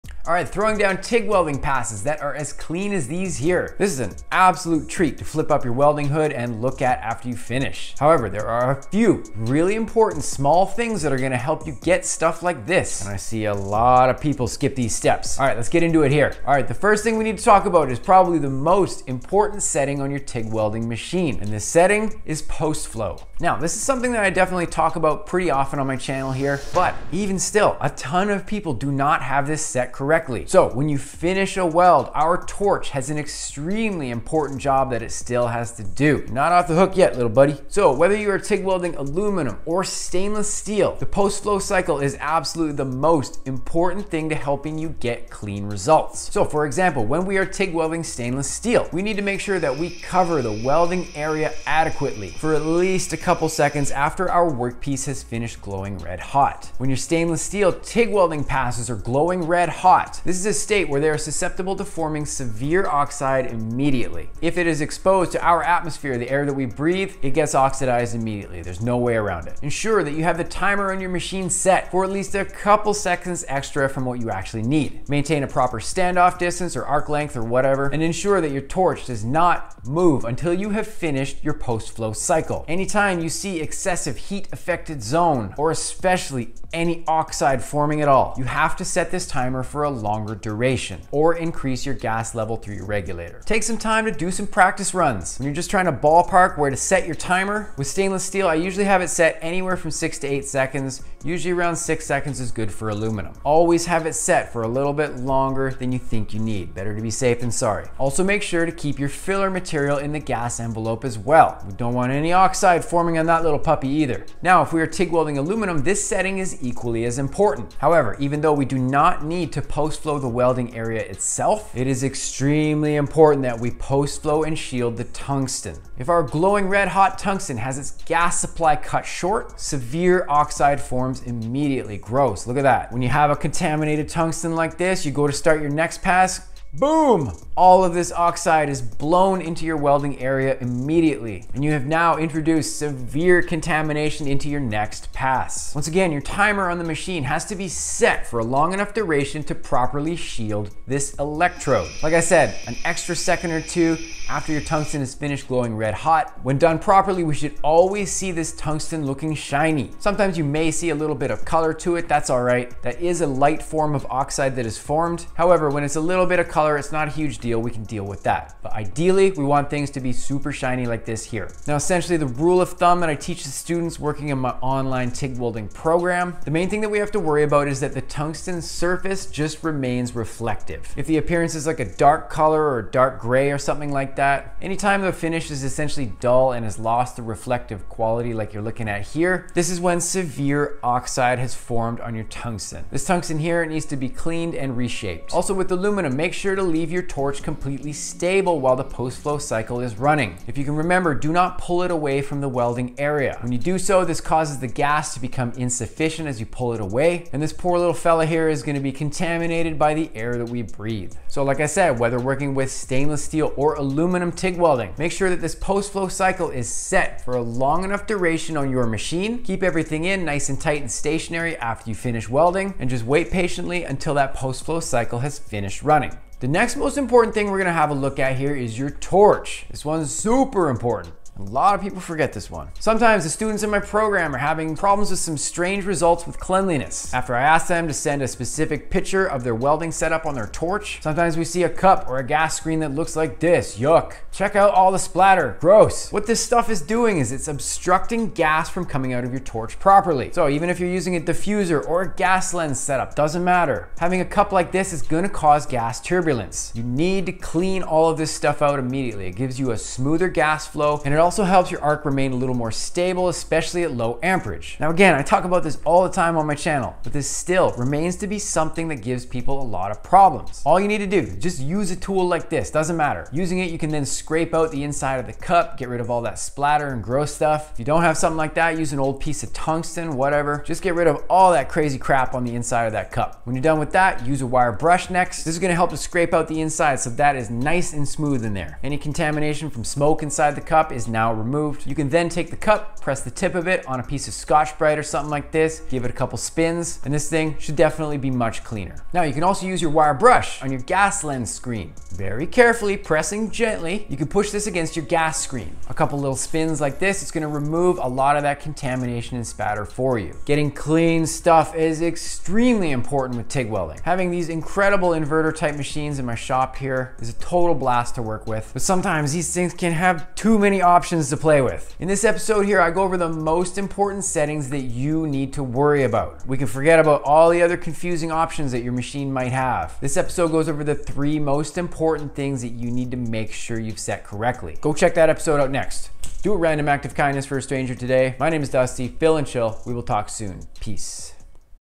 EASY tips to get cleaner tig welds 💎
Dirty tig welds?? Cmon my friend. Lets make sure we have done everything we can with our setups, and settings to get you some dimes that shine💎 And sometimes, a quick cleanup of torch parts, some settings and gas adjusting, and you're off and running. Grab a coffee on this episode, let's hang 🤙

🔥🔥🔥🔥BADASS Defiant Metal Welding gloves 🔥🔥🔥🔥